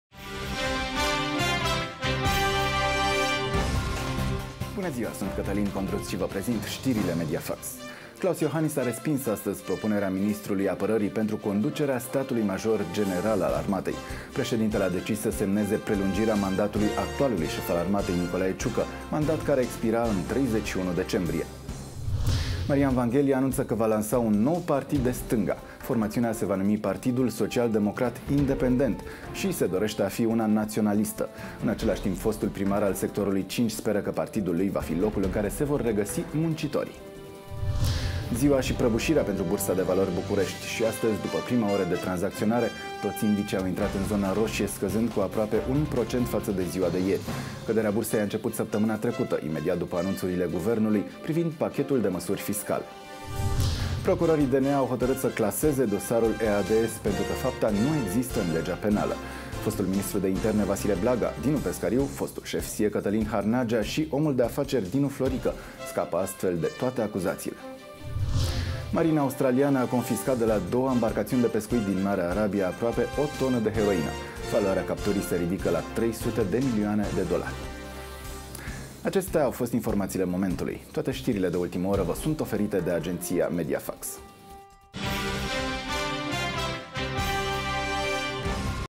Stiri Mediafax 28 Decembrie - Marian Vanghelie revine în politică
Știrile orei 17: Marian Vanghelie anunță că va lansa un nou partid de stânga. Formațiunea se va numi Partidul Social Democrat Independent și se dorește a fi naționalist. În același timp, fostul primar al sectorului 5 speră că partidul lui va fi locul în care se vor regăsi muncitorii.